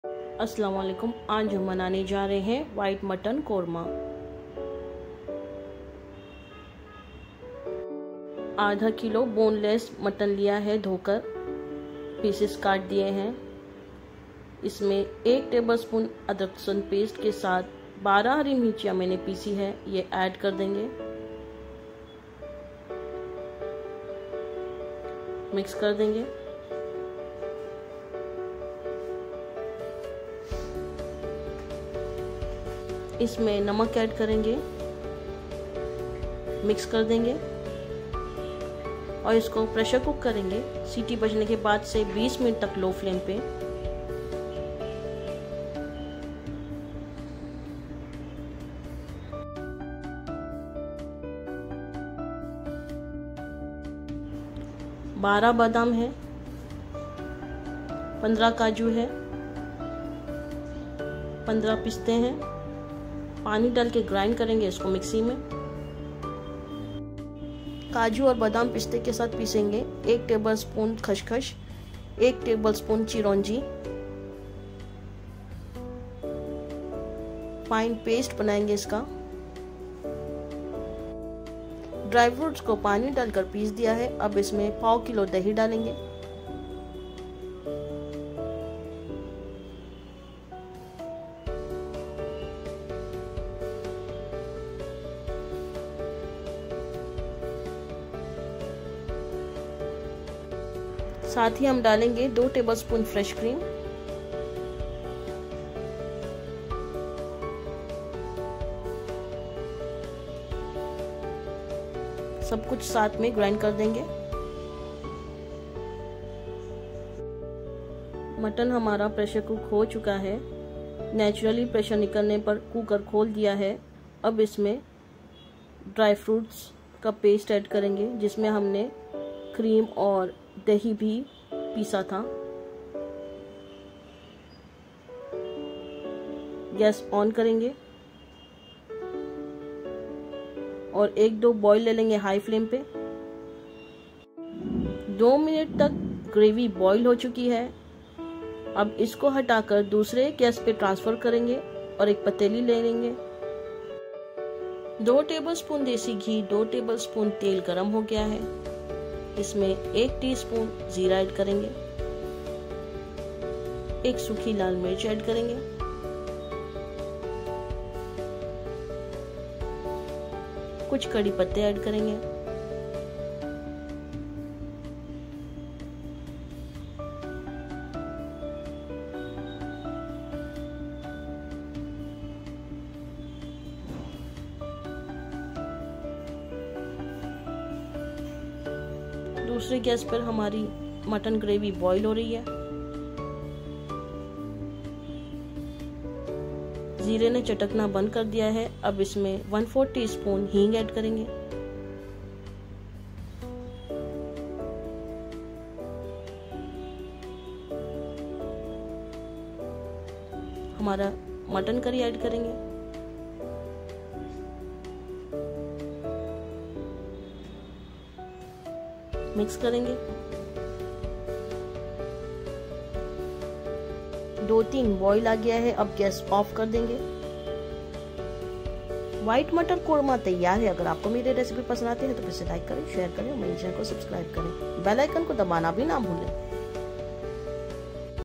White Mutton Korma (Bohra Style)/ Bakra Eid Special
Its an awesome recipe which is very delicious 😋.

Ingredients :-
1/2 kg washed boneless mutton
1 tbsp ginger garlic paste; Green chillies paste; salt;12 Almond; 15 Cashews; 15 Pistachios; 1 tbsp Poppy seeds; 1 tbsp Chironji seeds; 1/4 kg curd; 2 tbsp fresh cream
2 tbsp pure margarine (ghee)
2 tbsp oil
1 tsp Cumin seeds
1 dried Red chilli
Few curry leaves
1/4 tsp Asafoetida

Note - I added Poppy seeds & Asafoetida from my sight, Bohra's don't use them.